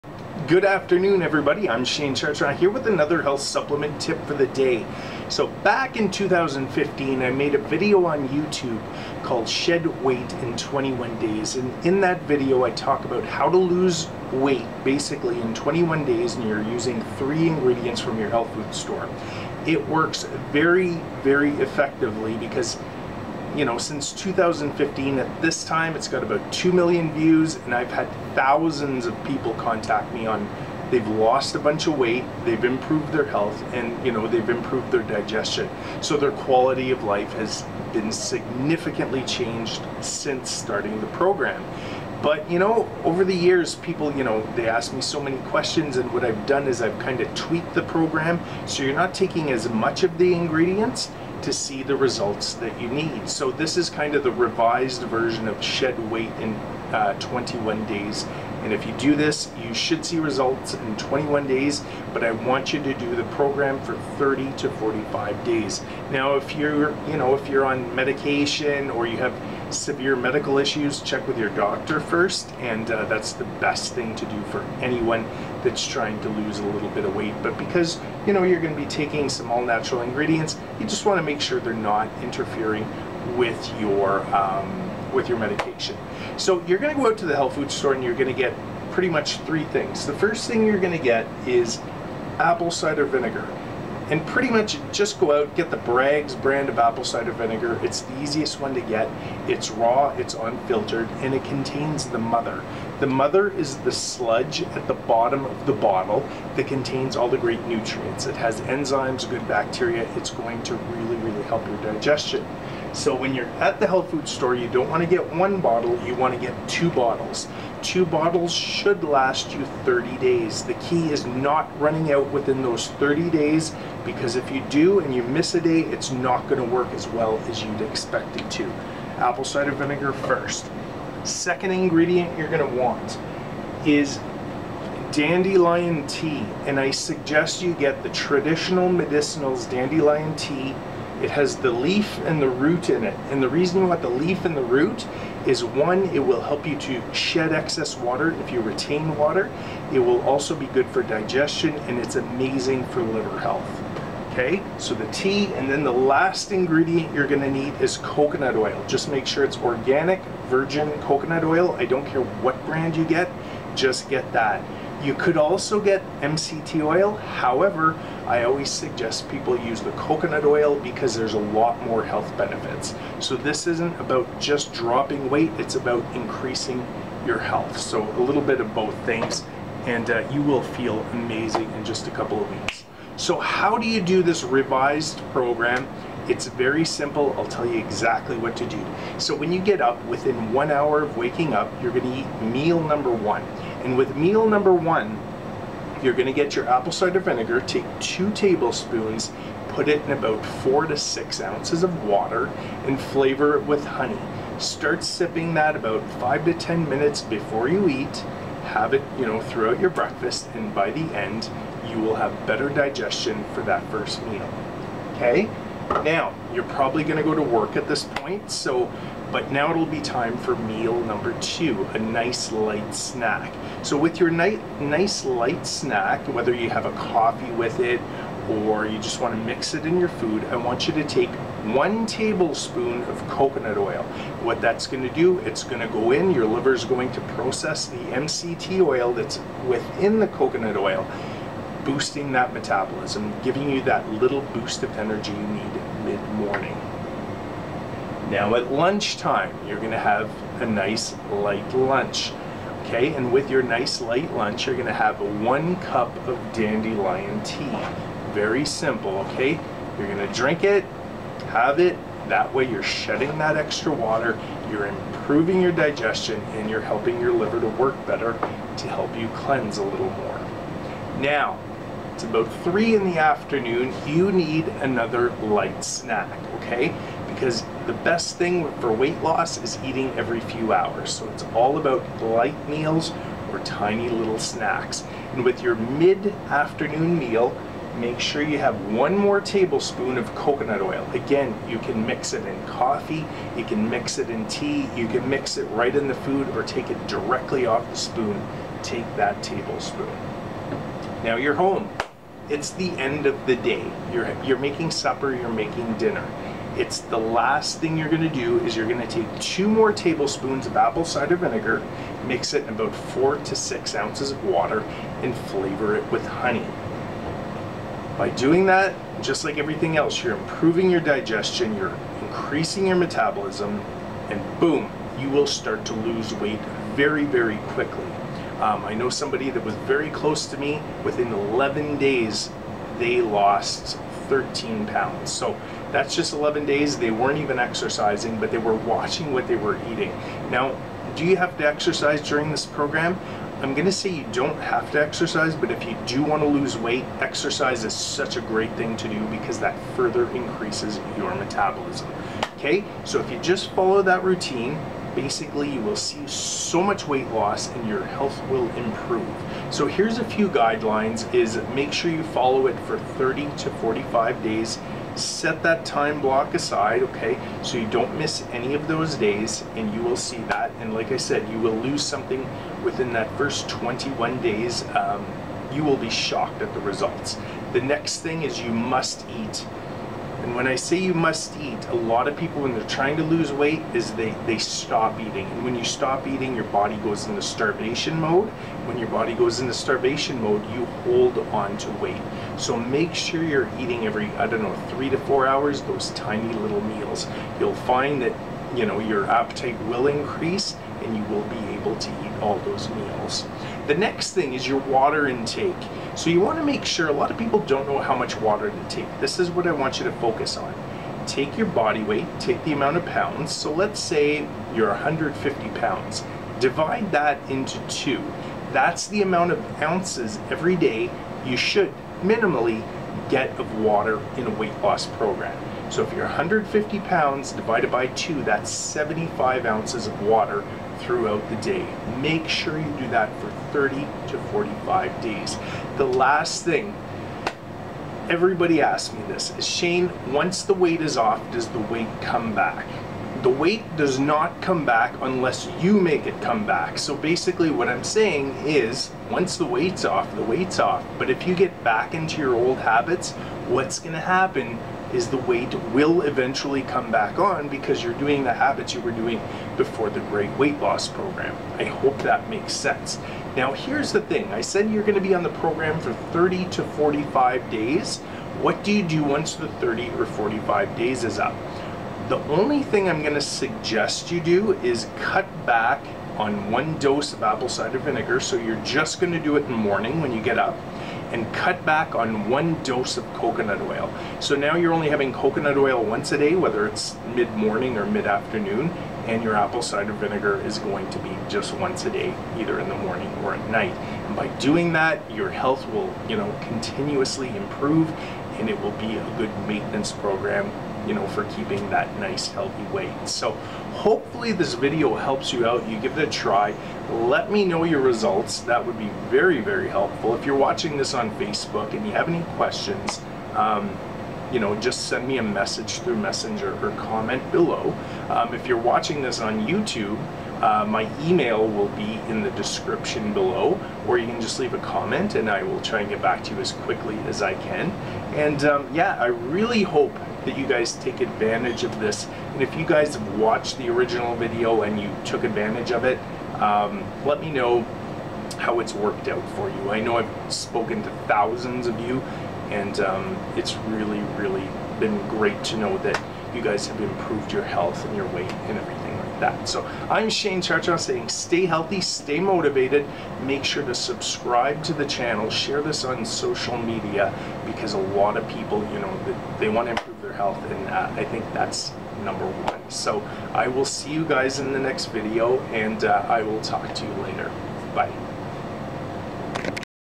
SHED WEIGHT IN 21 DAYS (Revised)
Was made and since then thousands of people have lost weight, improved their health and digestion.  This is a revised video outlining how the plan can be done.
Use the following supplements along with the meals as follows:
MEAL 1 (2 tbsp ACV)
MEAL 2 (COCONUT OIL 1 TBSP)
MEAL 3 (DANDELION TEA)
MEAL 4 (COCONUT OIL 1 TBSP)
MEAL 5 (2 TBSP ACV)

Guidelines:
-Follow for 30-45 days
-Drink Water
-Eat every 3-4 hours
Once the 30-45 days are up maintain with the maintance dose  (explained in video)